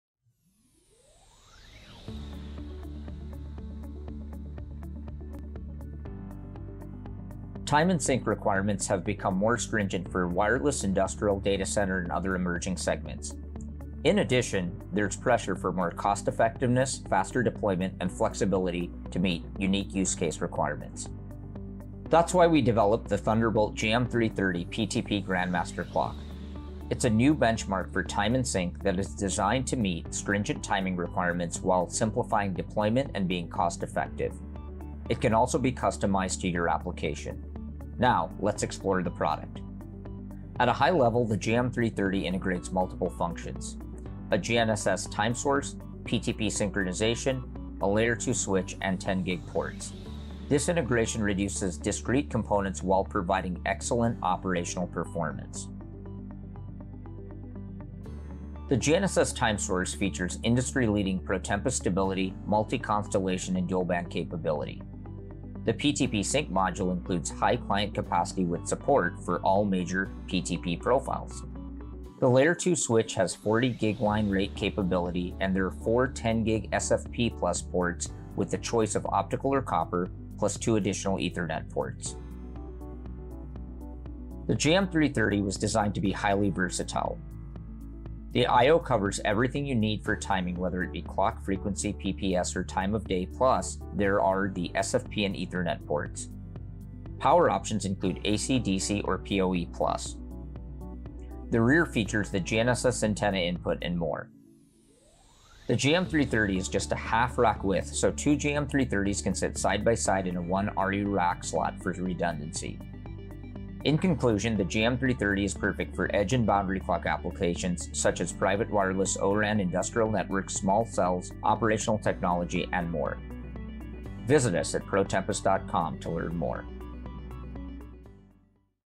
Announcing: Thunderbolt™ GM330 PTP Grandmaster Clock with Integrated Functions
The Thunderbolt GM330 PTP Grandmaster Clock integrates GNSS timing, PTP synchronization, L2 switch and 10G ports to create cost and deployment time savings. This product features superior performance, small form factor, security, comprehensive management, and versatility.

This product is ideal for addressing time and sync needs for:
-Private 5G
-ORAN
-Smart Grid
-Industrial
-Broadcast
-Transportation
-And More...